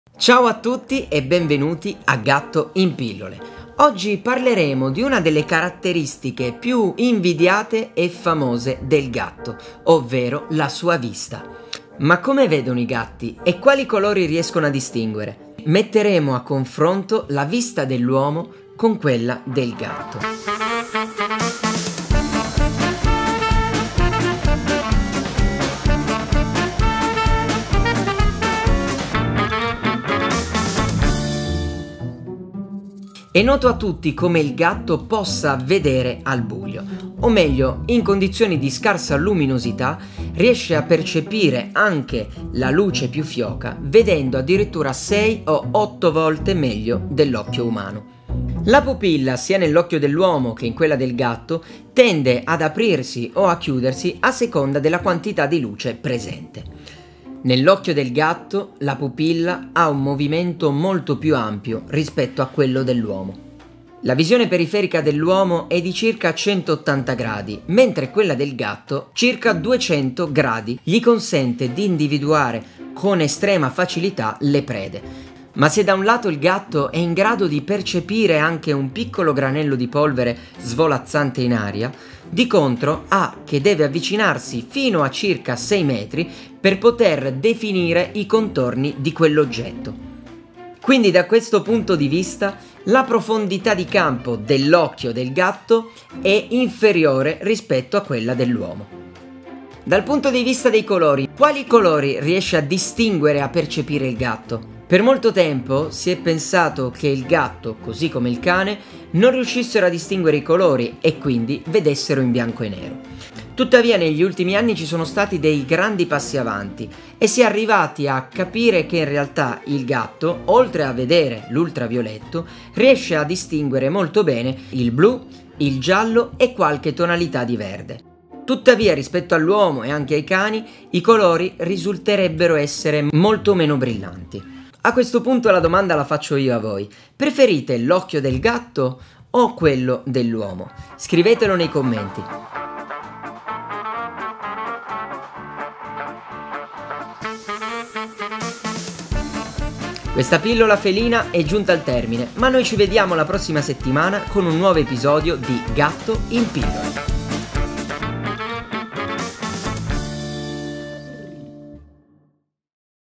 Come VEDE il Gatto?🐱 entriamo nei suoi occhi!
Nel video non ho voluto trattare scientificamente la l'argomento, ma mostrarvi con l'aiuto delle immagini e in parole semplici come potrebbe vedere il Gatto.

📚IL mio Libro "FATTI I GATTI TUOI"  in tutte le librerie o su AMAZON